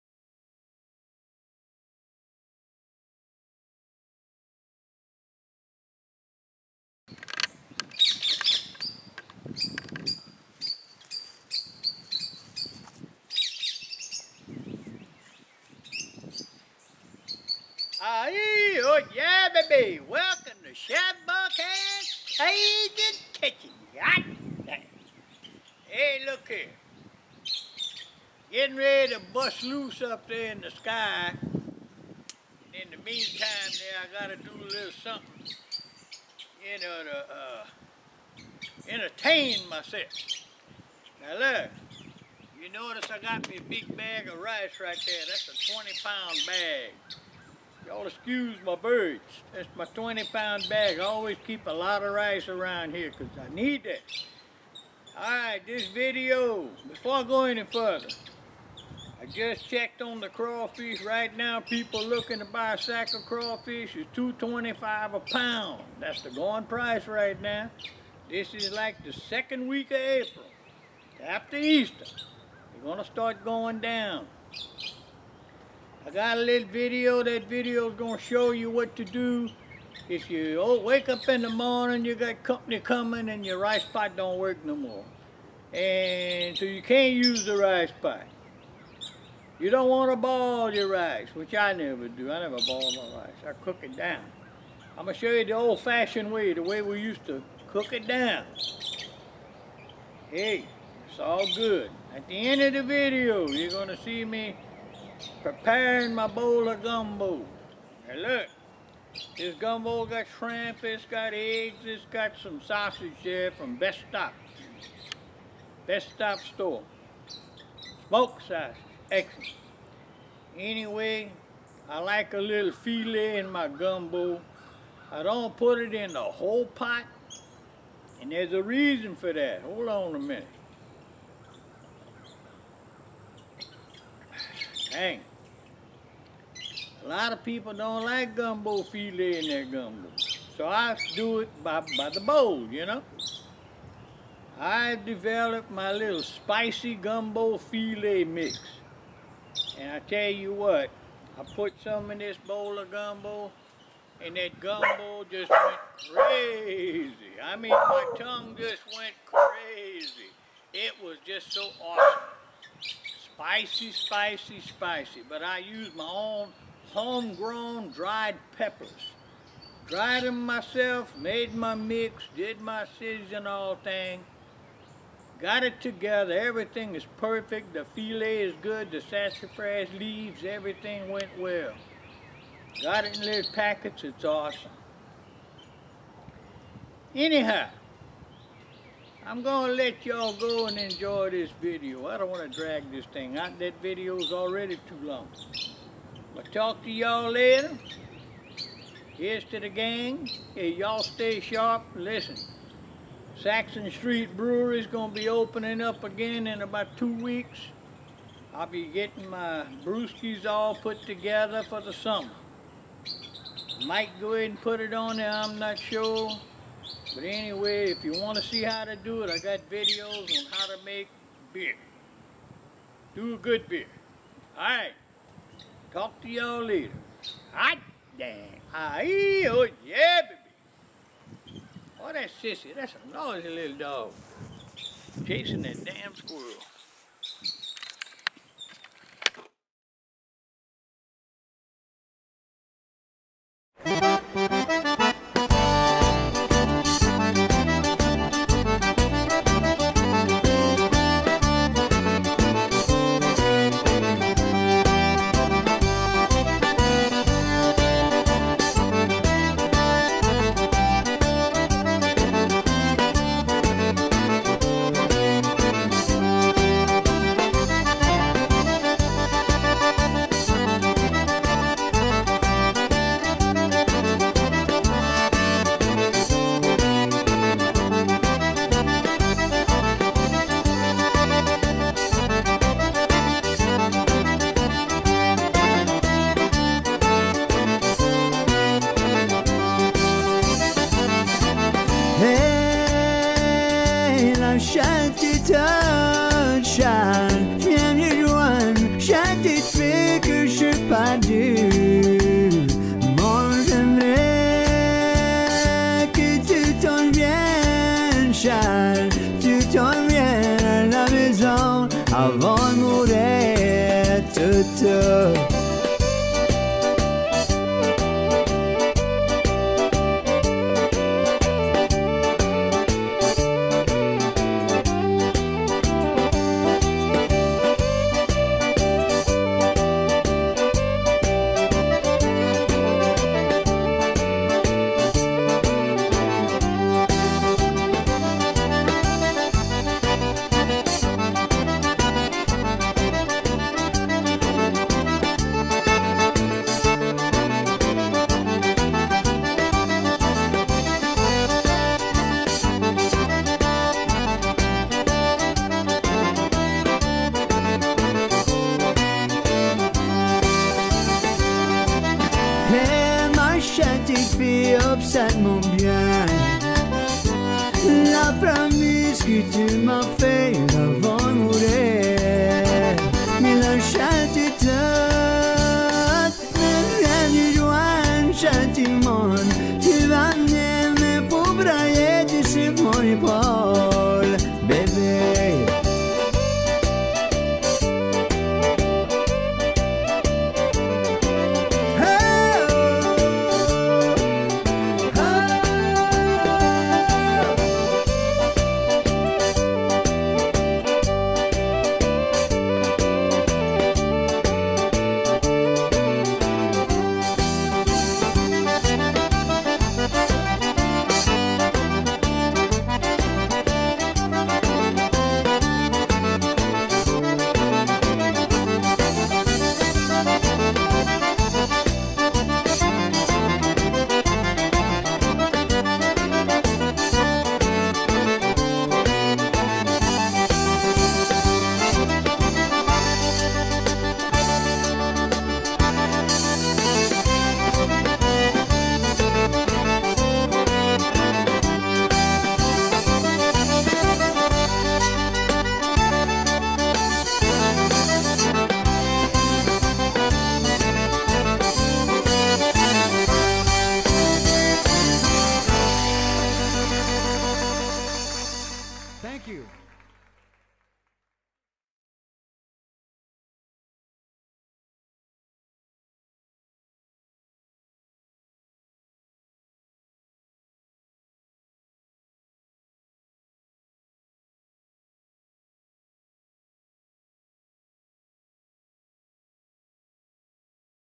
Rice Cooked The Old Fashioned Cajun Way.
Join chef Bourque as he prepares his old fashioned rice, and his bowl of gumbo using his Spicy Gumbo File'. Aieee!